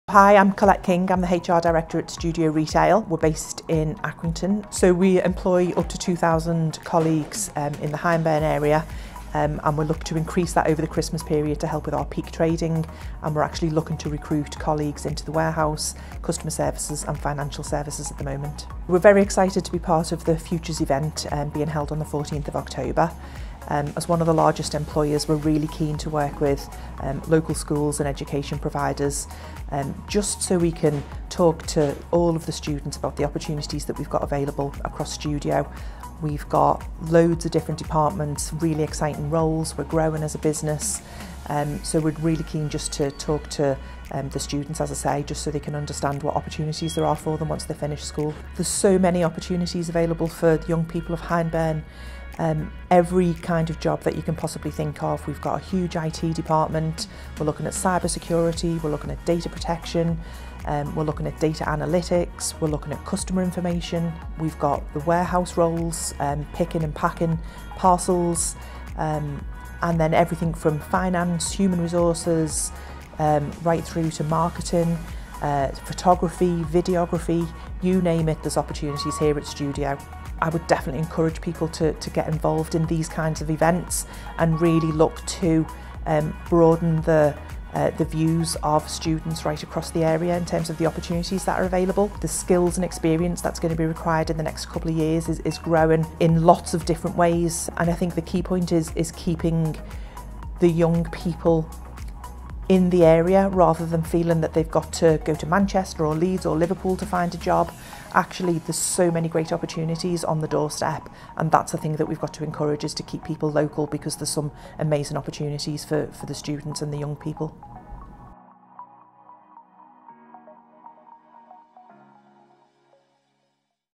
Studio Futures Event
The event in Accrington Town Hall will see hundreds from the secondary schools in Hyndburn meet local businesses including Studio Retail.

Listen to what HR Director Collette King had to say.